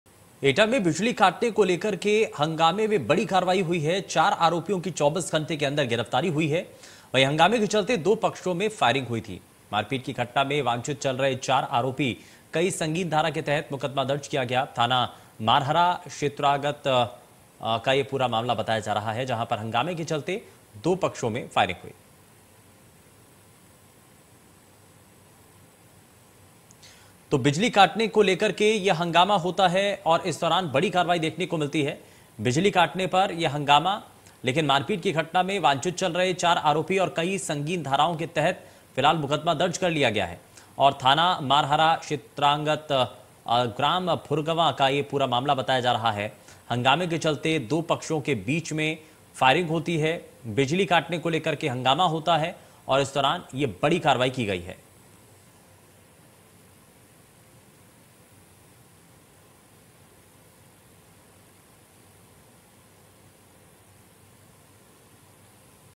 Etah:बिजली काटने को लेकर हुए हंगामा में बड़ी कार्रवाई, चार आरोपियों की 24 घंटे के अंदर हुई गिरफ्तारी
Etah:बिजली काटने को लेकर हुए हंगामा में बड़ी कार्रवाई, चार आरोपियों की 24 घंटे के अंदर हुई गिरफ्तारी | India News UPUK